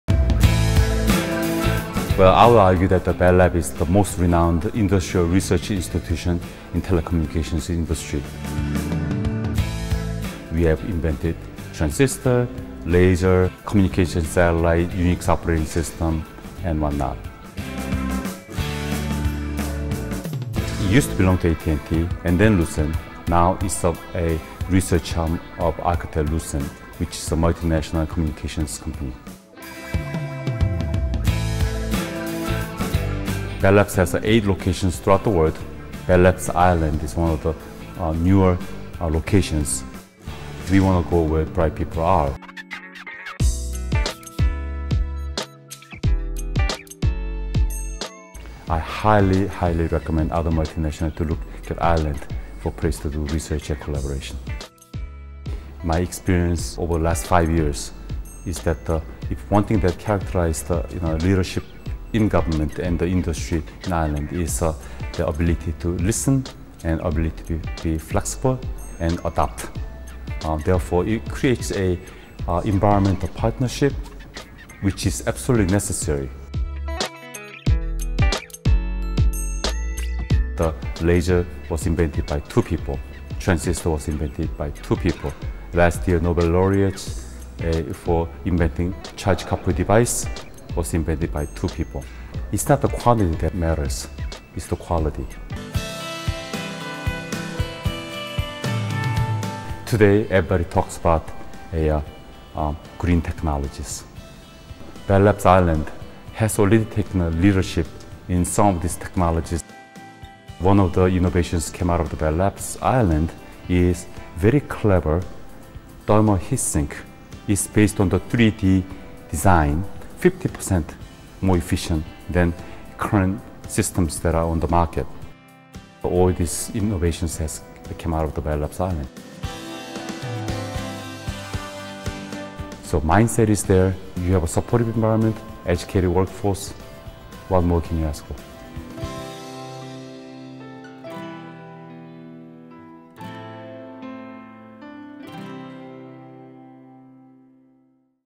Interview with Dr. Jeong Kim, President, Bell Labs
Dr. Jeong Kim, President, Bell Labs